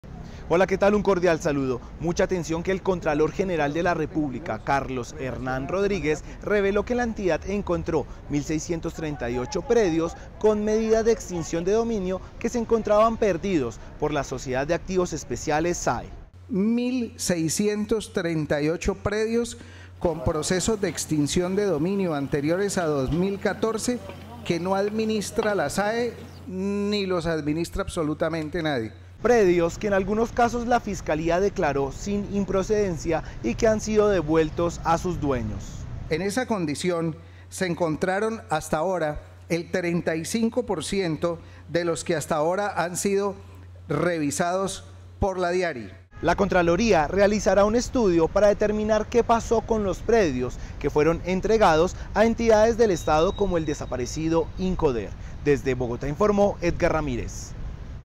Contraloría encuentra 1.638 predios perdidos por la SAE
Contraloría encontró 1638 predios con procesos de extinción de dominio anteriores a 2014 que no administra la SAE ni nadie, El organismo de control precisó que entregará a la Fiscalía y a la SAE la base de datos de estos predios que se consideraban perdidos, para que se coordinen y los busquen, de forma tal que se materialicen.